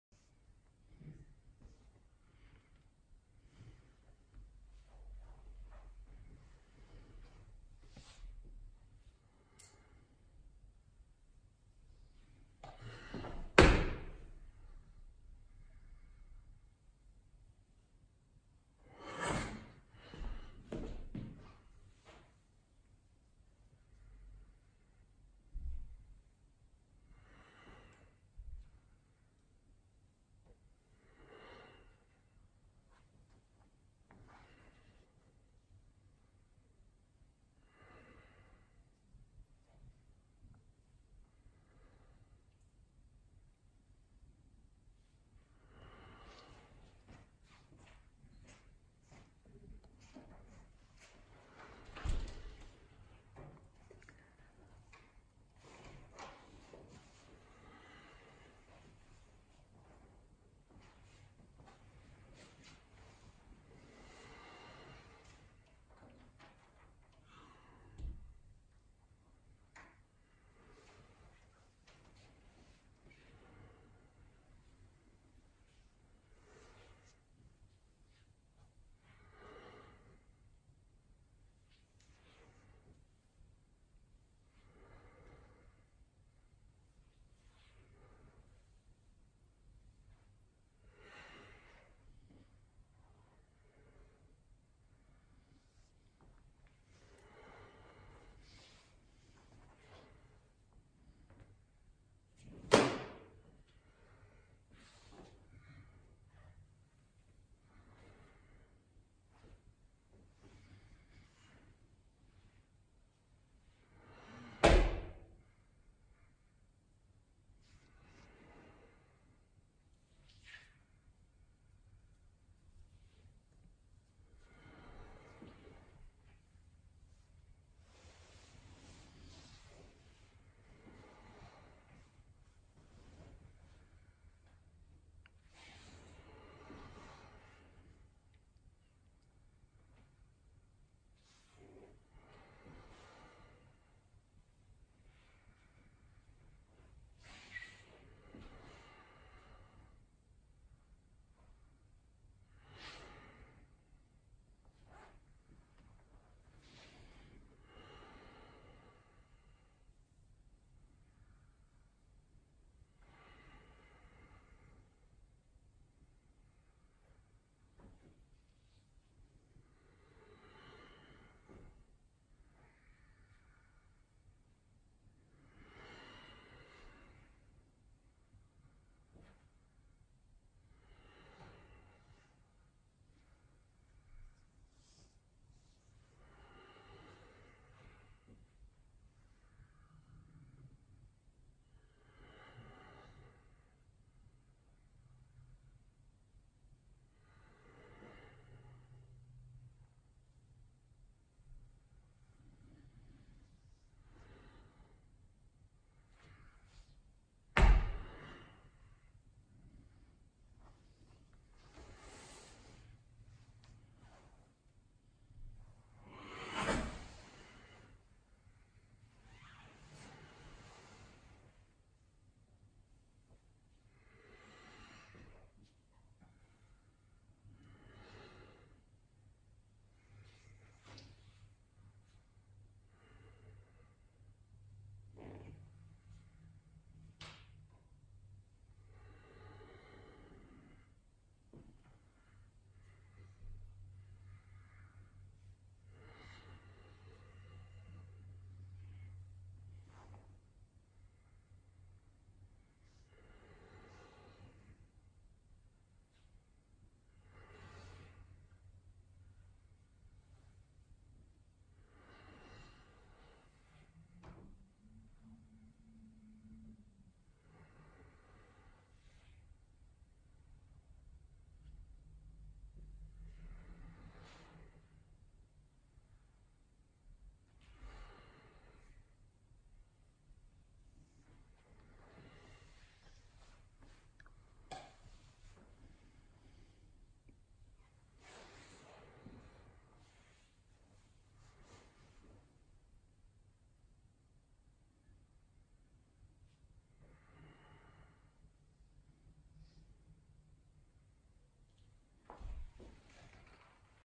Chen style 74-form (Lao Jia Li Yu), movements 48-63
Master Wu Yuping, co-founder of the Yangshuo Traditional Tai Chi School demonstrates the Chen style 74-form, from movements 48 (Jade Maiden Working Her Loom) to 63 (Patting the Horse’s back)